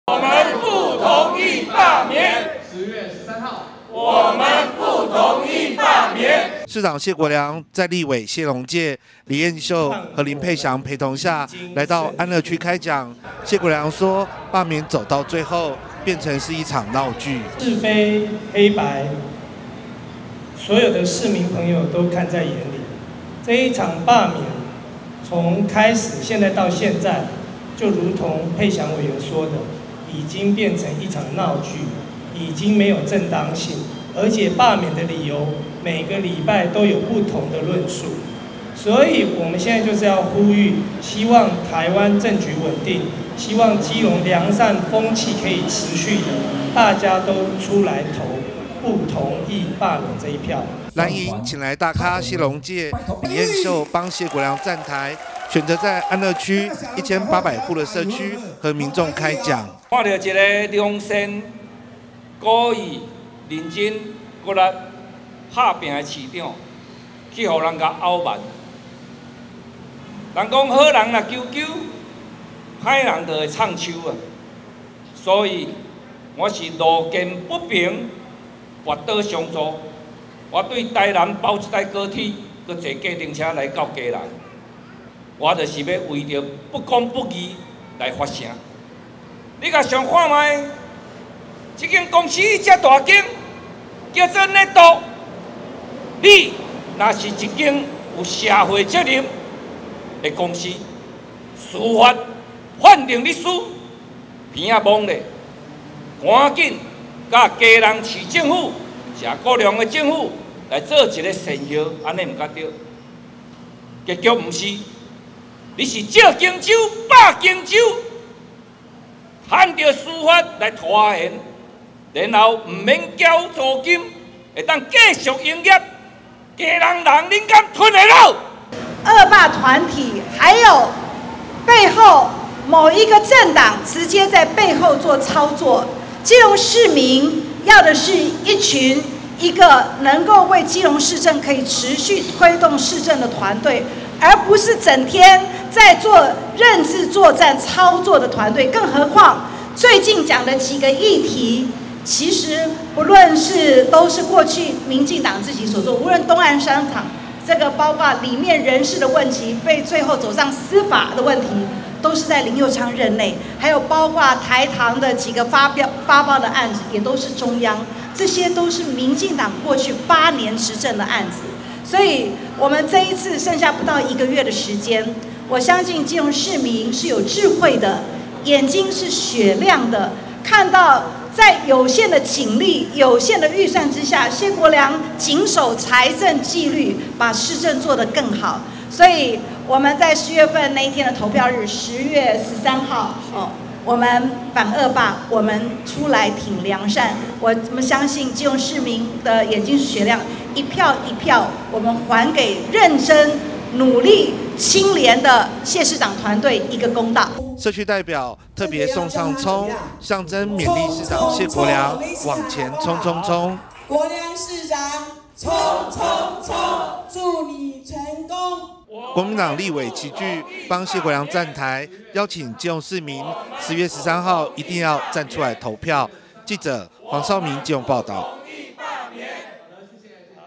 力挺謝國樑｜謝龍介｜力挺謝國樑！立委謝龍介「台語8字」拔刀相助
(中嘉新聞記者黃紹銘/基隆採訪報導) 距離罷免投票，剩下不到一個月，基隆市長謝國樑15日在立法委員謝龍介、李彥秀、林沛祥等人陪同下，在安樂區城上城社區展開宣講活動。謝龍介表示，他為了力挺認真努力的謝國樑，搭高鐵轉計程車來到基隆，他強調好人如果沉默、壞人就會囂張，目的就是路見不平、拔刀相助，呼籲市民10/13投下不同意罷免票。